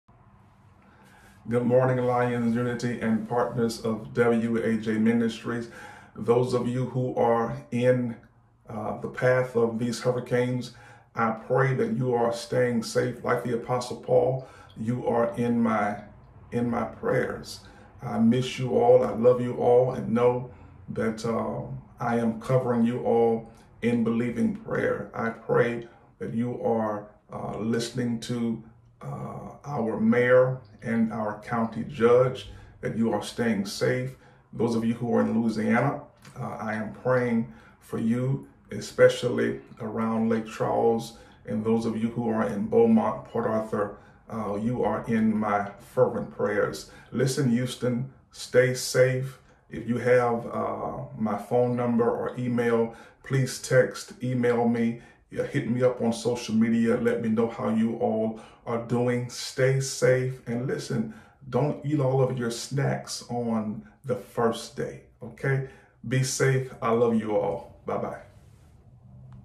Praying For You
Please listen to this message from Pastor Jordan. We are praying for you all.